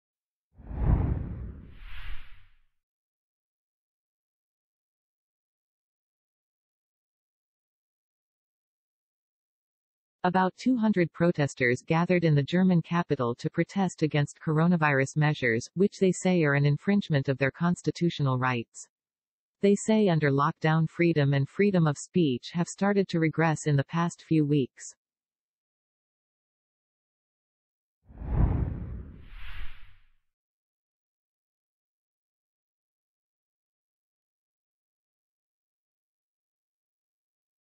Berlin march against lockdown measures
About 200 protesters gathered in the German capital to protest against coronavirus measures, which they say are an infringement of their constitutional rights.

They say under lockdown freedom and freedom of speech have started to regress in the past few weeks.